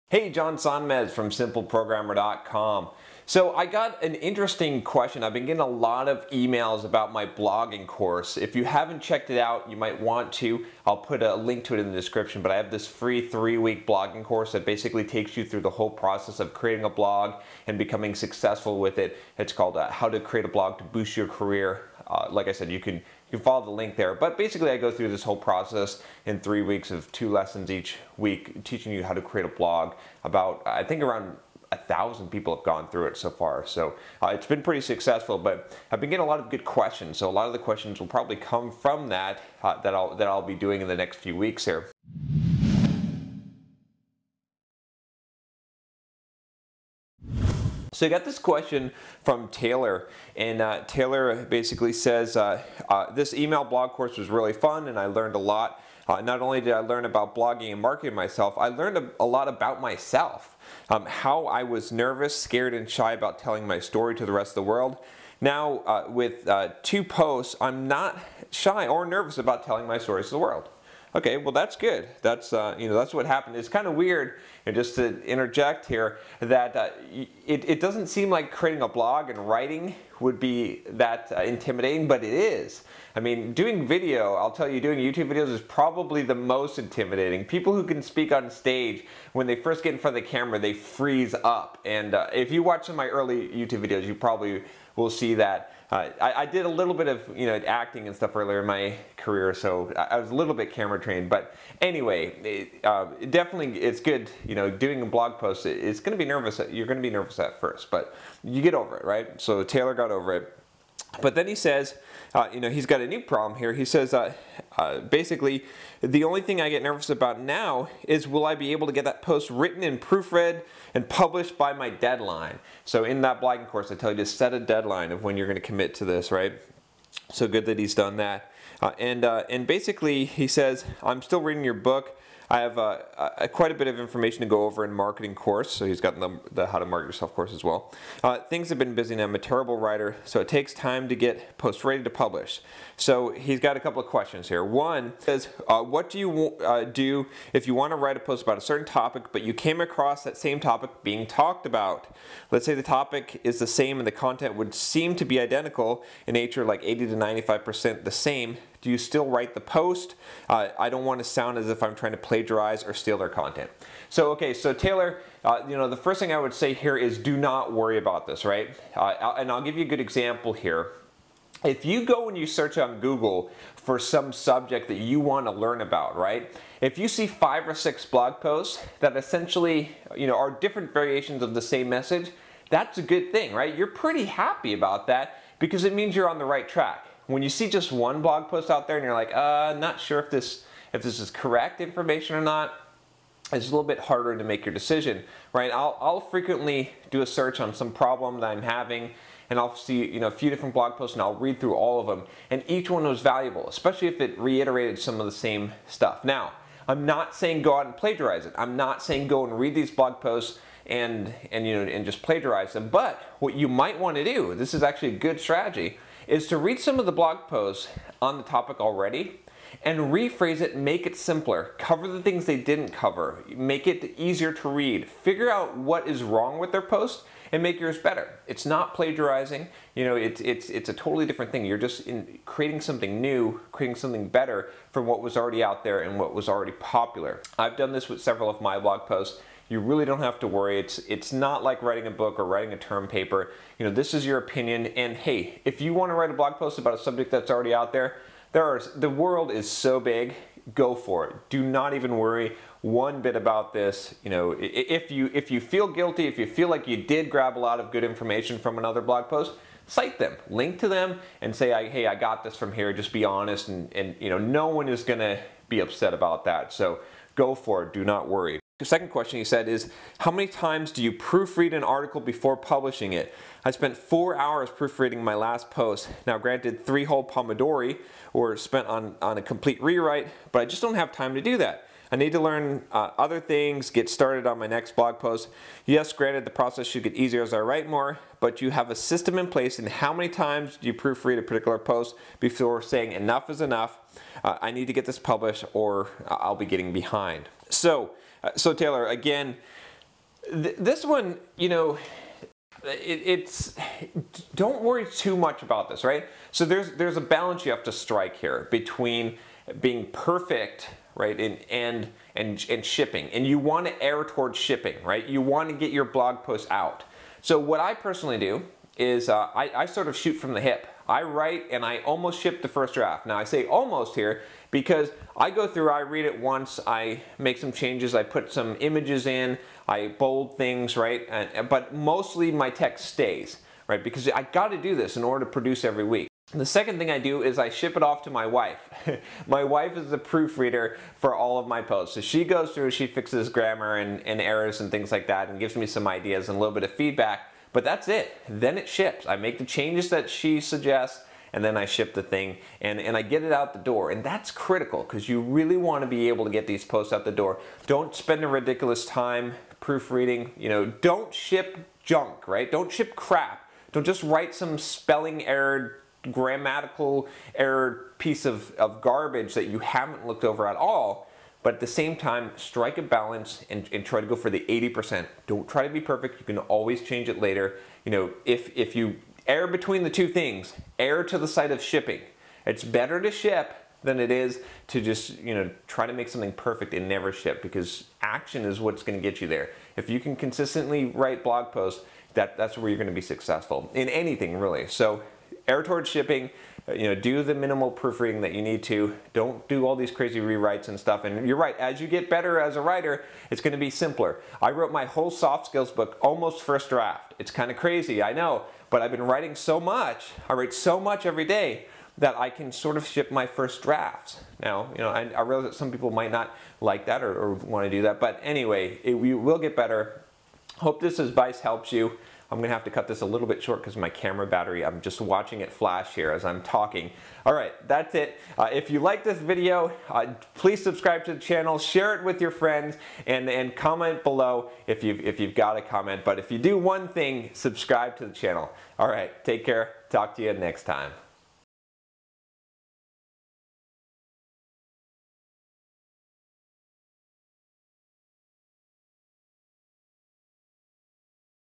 Is It Ok To Copy Someone Else's Blog Post?
Learn what to do if your topic is already covered and how to get your blog post published fast!